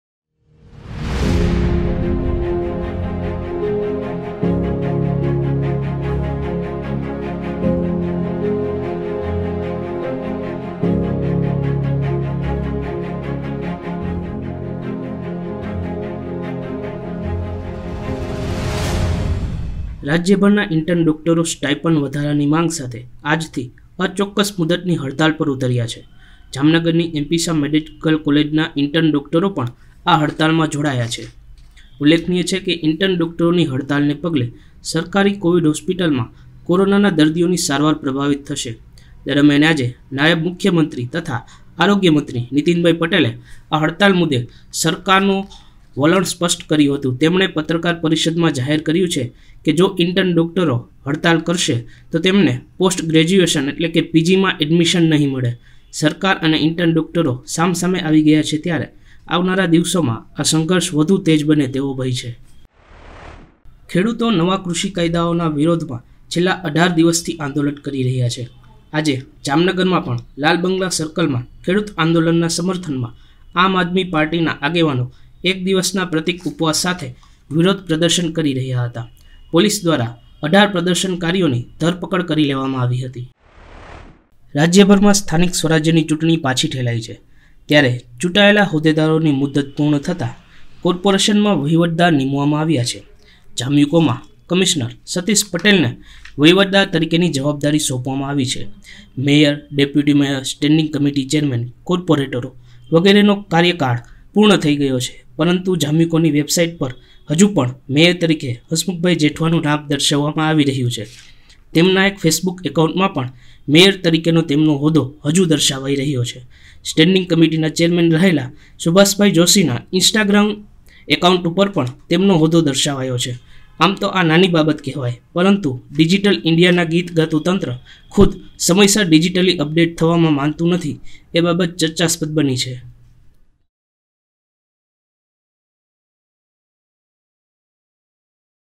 નોબત ન્યૂઝ ફ્લેશ 14-12-2020 :- હડતાલ કરનારા ઈન્ટર્ન ડોક્ટરોને પીજી માં એડમીશન નહી મળે
Voiceover: Aditya Jamnagari